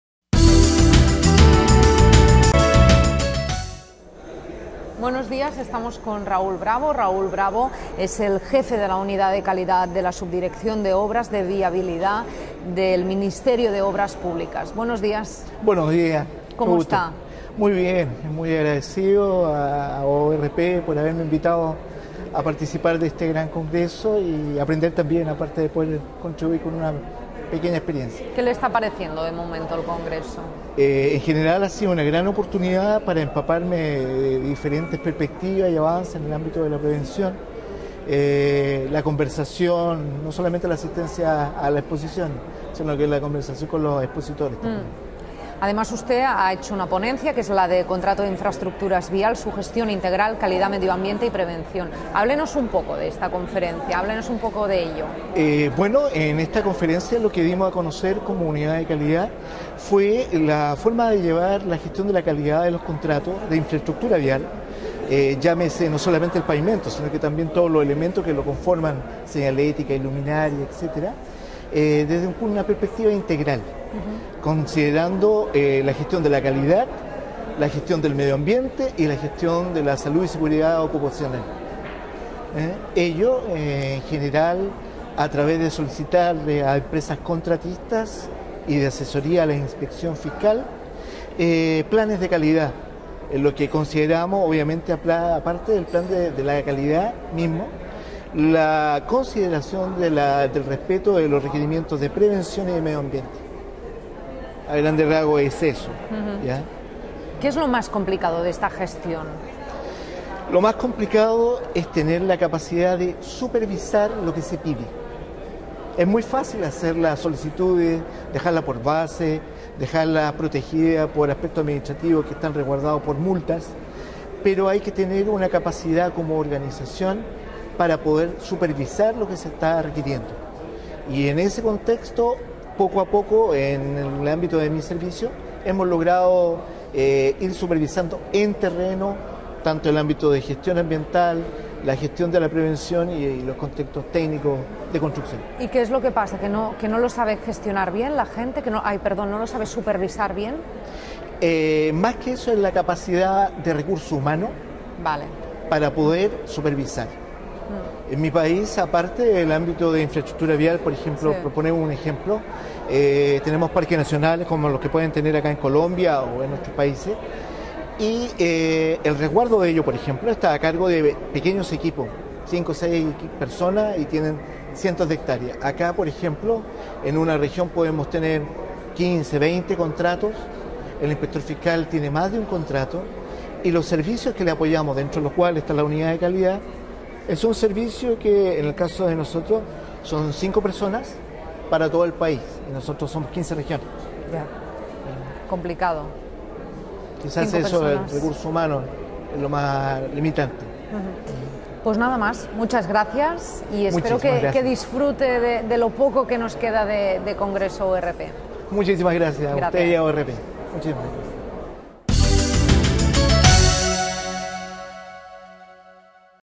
Entrevista - Raúl Bravo
Raúl Bravo, jefe UC – SOV – MOP, participó en el XVI Congreso Internacional ORP que se celebró en Cartagena de Indias, Colombia.